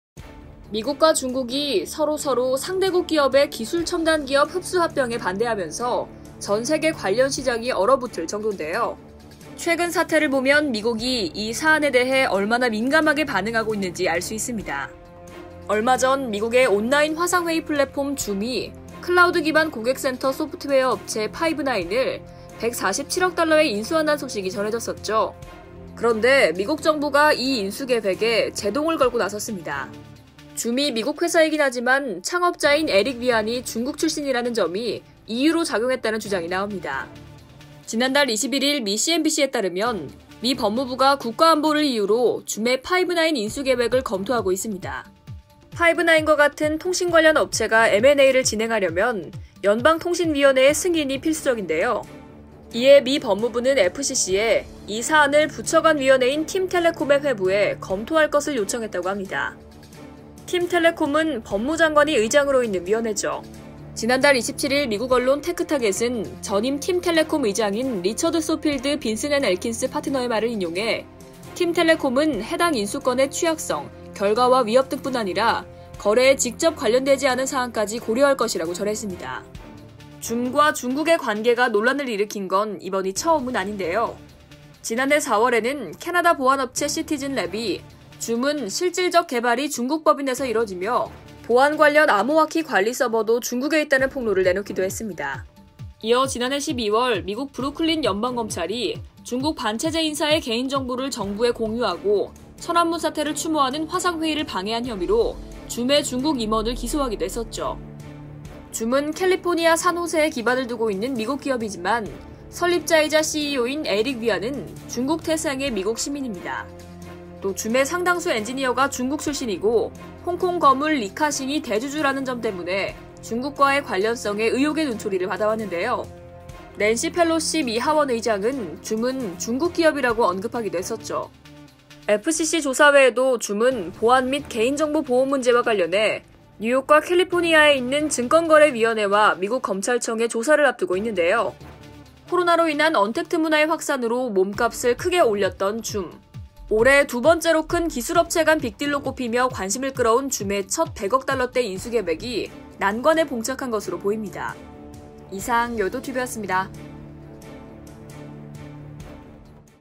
“겉만 미국기업이지 속은 온통 빨간색“ 美, 17조 딜 안돼! [여의도튜브/머니투데이방송]
미국과 중국이 서로서로 상대국 기업의 기술첨단기업 흡수합병에 반대하면서 전세계 관련 시장이 얼어붙을 정도인데요, 최근 사태를 보면 미국이 이 사안에 대해 얼마나 민감하게 반응하고 있는지 알 수 있습니다.

아나운서: 선소연
영상콘텐츠: 강주희
기사콘텐츠: 선소연
기획: 강인형
총괄: 정미경
제작: MTN 머니투데이방송